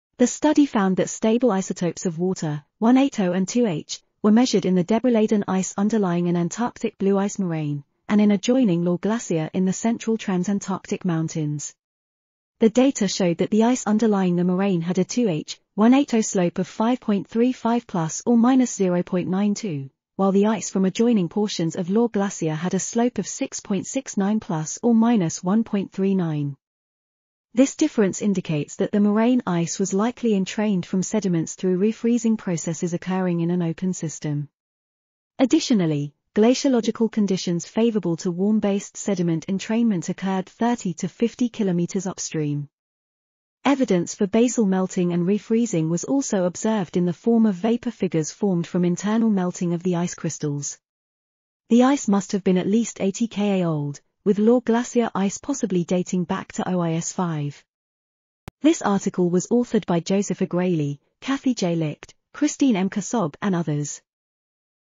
Warm-based basal sediment entrainment and far-field Pleistocene origin evidenced in c... | RTCL.TV
### Keywords ###

### Article Attribution ###
Title: Warm-based basal sediment entrainment and far-field Pleistocene origin evidenced in central Transantarctic blue ice through stable isotopes and internal structures
Authors: JOSEPH A. GRALY, KATHY J. LICHT, CHRISTINE M. KASSAB, BROXTON W. BIRD ,and MICHAEL R. KAPLAN

### Video Timestamps ###
0:00:00 - Summary
0:00:58 - Title
0:01:04 - End